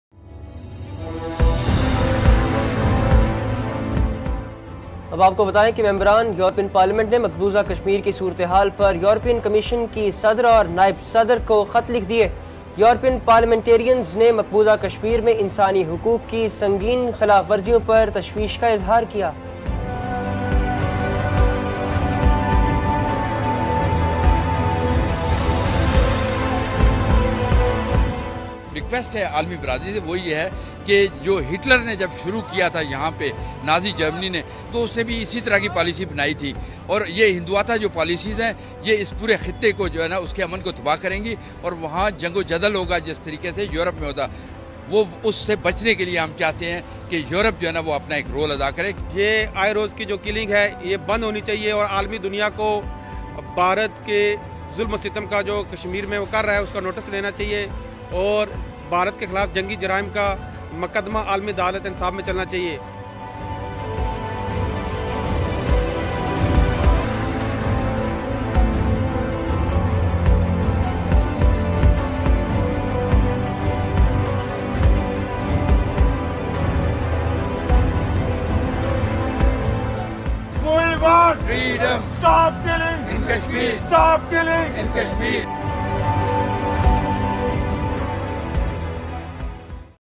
EU PARLIAMENTARIAN LETTER ON IIOJK
Sixteen European lawmakers have addressed a letter  to European Commission President Ursula von der Leyen and Vice President Josep Borrell, urging European Union to raise its voice on human rights violations in IIOJK and take action.

This letter is yet another demonstration of the continuing global censure of the ongoing human rights violations and humanitarian crisis in IIOJK.

India continuously peddling false propaganda in futile attempts to push the sham narrative of so-called ‘normalcy’ in IIOJK".

Indian actions has only increased after "egregious violations of HR and fundamental freedoms" in the wake of India's "illegal and unilateral" actions to strip IIOJK of its special autonomy on August 5, 2019.